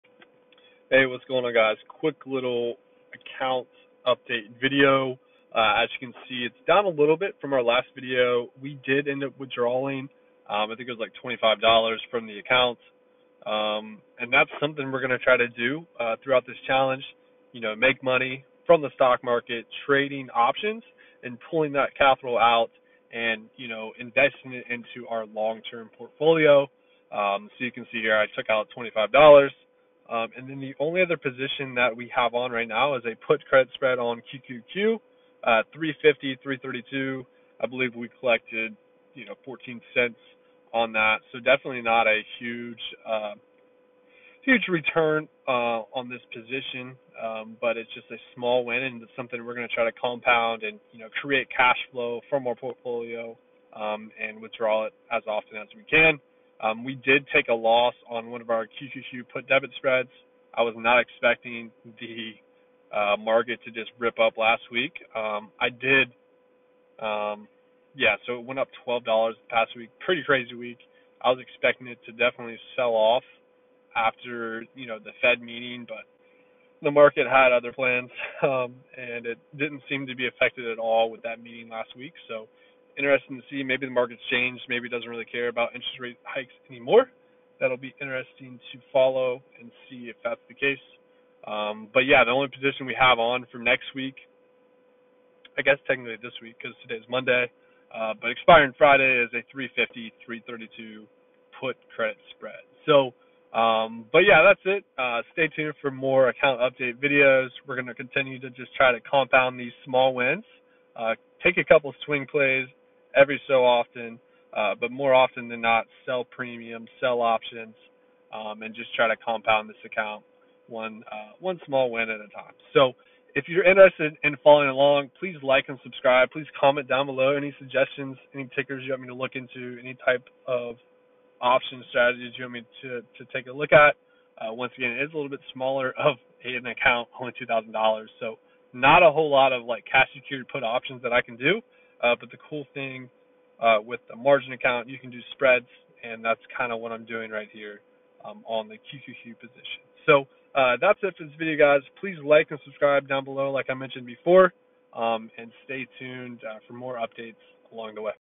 Robinhood Account Challenge Update 📈📉
Robinhood Small Account Update

Sorry for the bad audio, my car Bluetooth must’ve took over on the mic lol